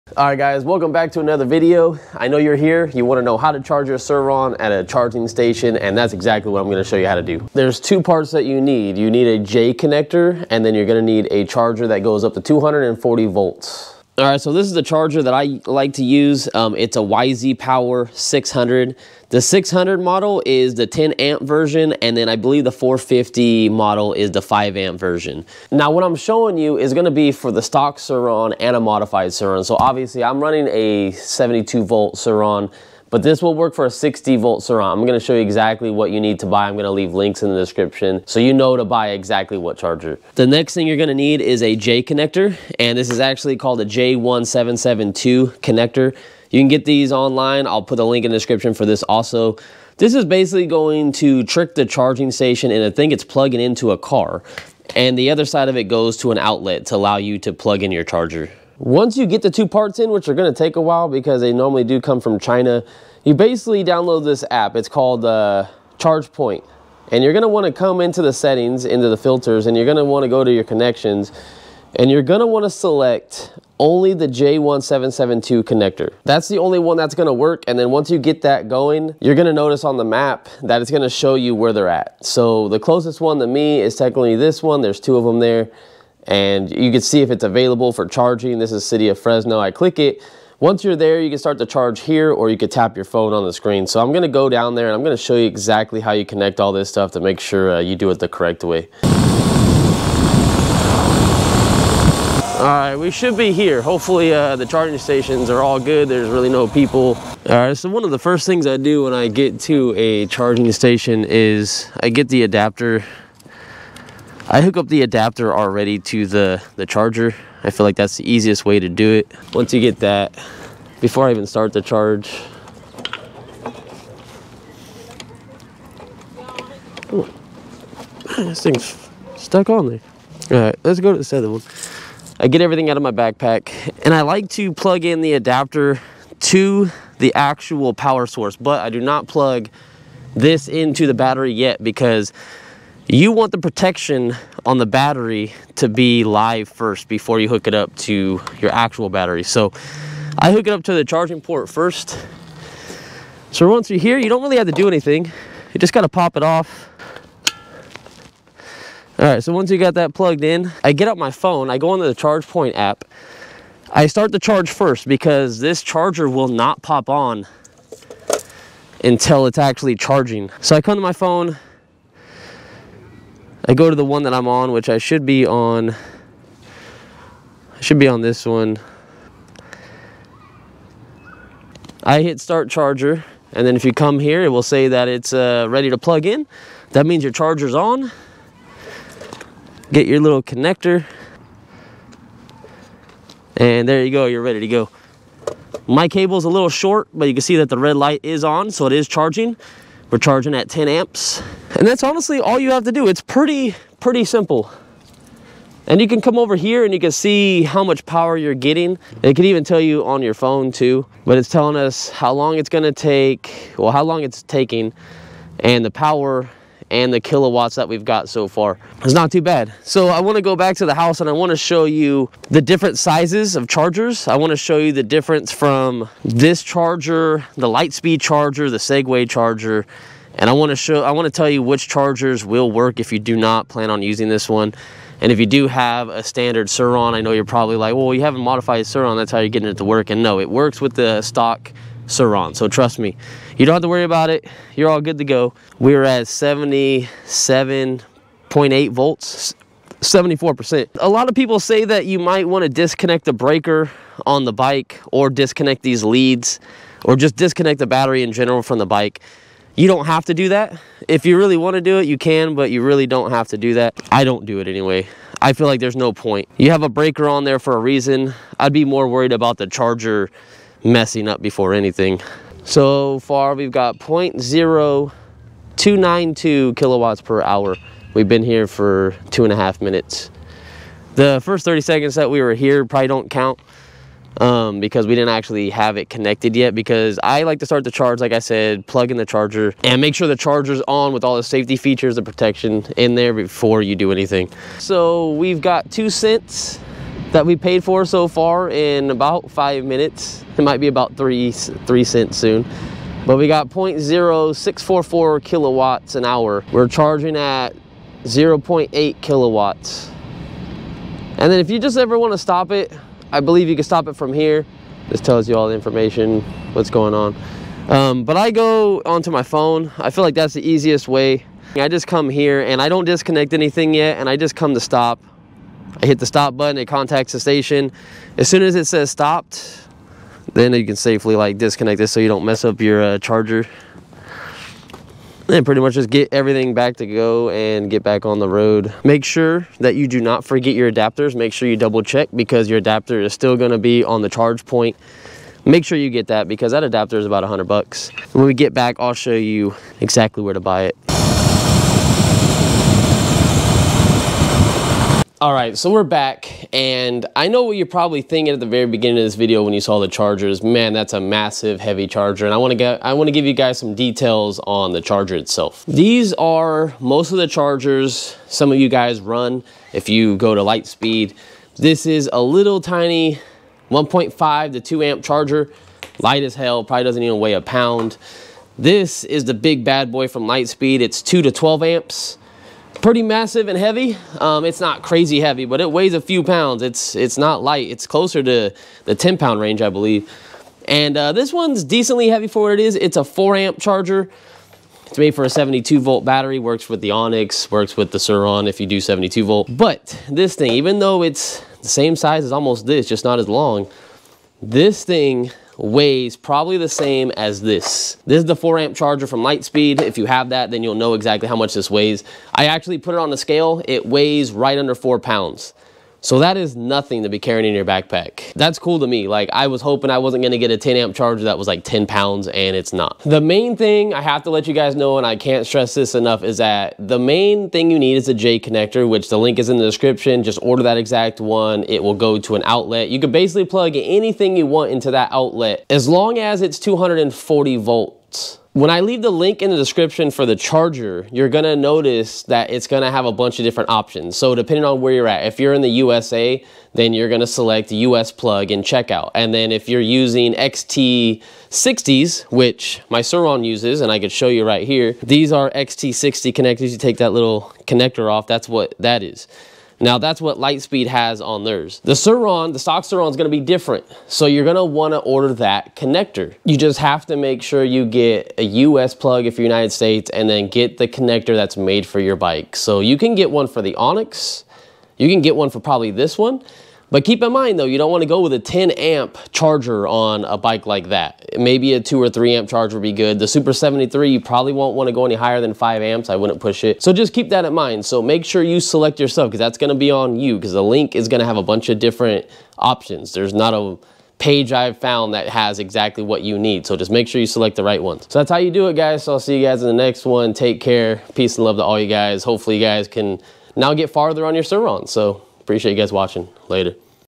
Sur Ron & Segway EV Charging Station // HOW TO with LINKS!
MAIN ADAPTER YOU MUST BUY!

If you live in the United States, Order USA plug and I highly suggest ordering the charger with a XT60 Connector if ordered from aliexpress (yzpower chargers). Use the adapters listed below for your bike Setup to go from xt60 to (you're bikes charging port)

60v SURRON Battery Chargers (original battery)

SUR RON 60v 4amp (amazon):
SUR RON 60v 5amp:
SUR RON 60v 10amp:

72v SURRON Battery Chargers (upgraded batteries)

SUR RON/ONYX 72v 5amp
SUR RON/ONYX 72v 10amp
SUR RON/ONYX 72v 5amp (high end)

SUPER73 BIKE COMPATIBLE CHARGERS
Super 73 (S2, R, & RX) 7.5amp:

COMPATIBLE OEM EV CHARGERS:
SEGWAY x260 Stock Charger - 10 amp
Onyx RCR Stock Charger - 5amp

NOT COMPATIBLE EV CHARGERS:
Onyx SFX Charger - 10 Amp
Lite Speed Bikes Charger - adjustable 2 to 12 Amp
Lite Speed Bikes Charger - 2 amp
Lite Speed Bikes Charger - 4 amp
SUR RON Stock Charger - 10 Amp

HUGE thank you to JOHN ANGEL for the help!!!